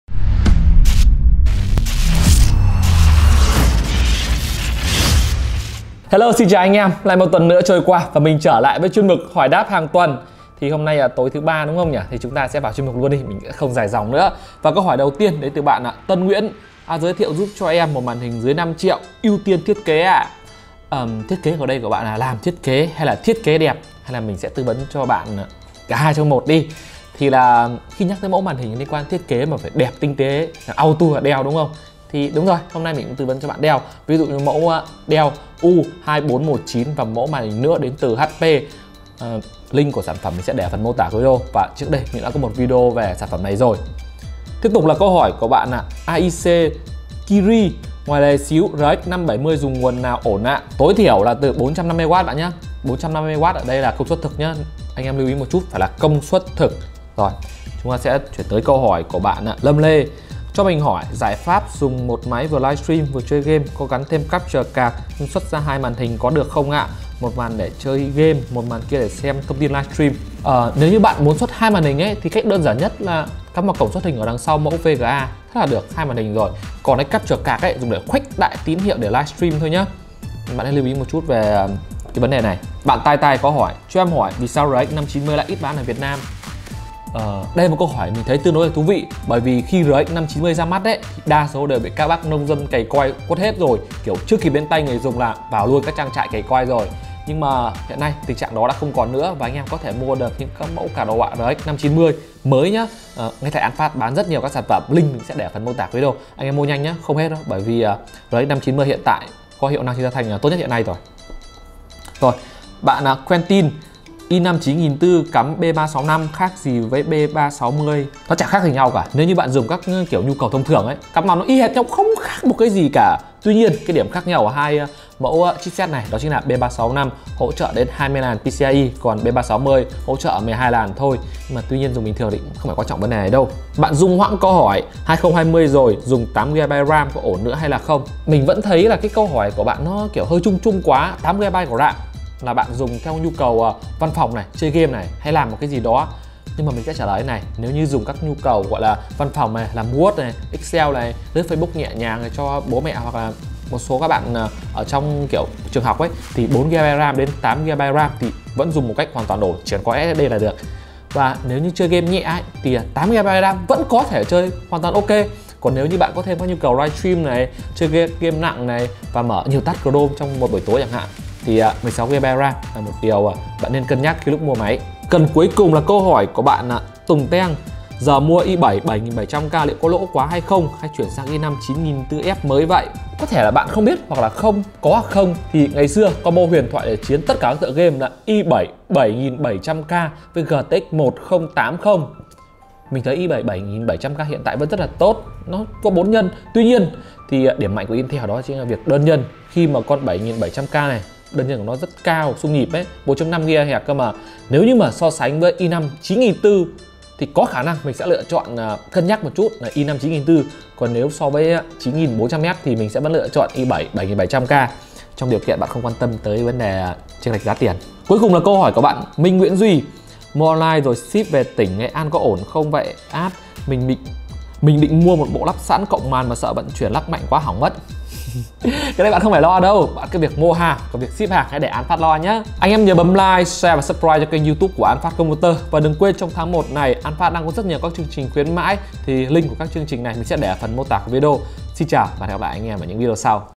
Hỏi đáp #10 - Chọn mua i7 7700k cũ hay i5 9400F mới | An Phat PC
SIÊU THỊ MÁY TÍNH AN PHÁT